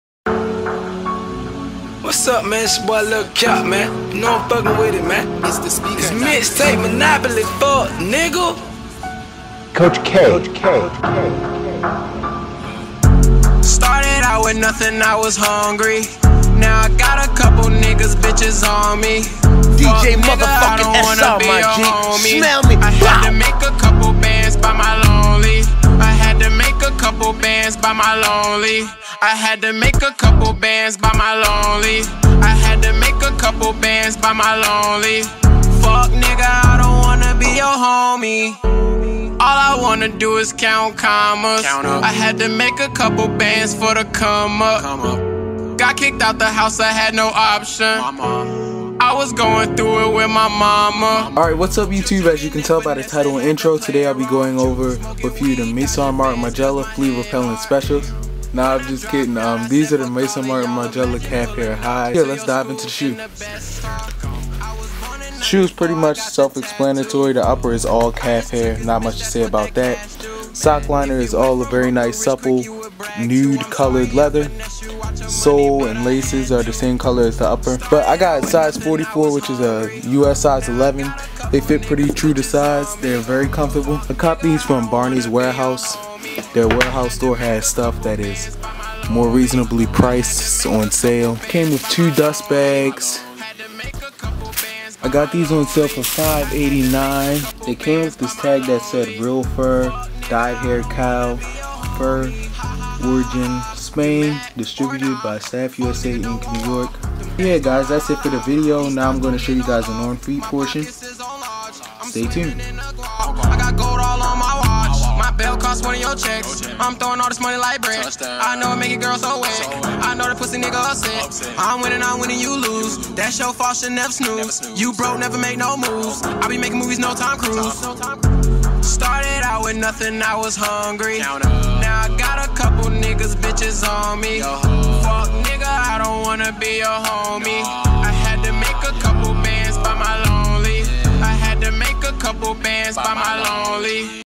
Maison Martin Margiela Calf Hair High Sneaker
Quick Overview of one of my recent pickups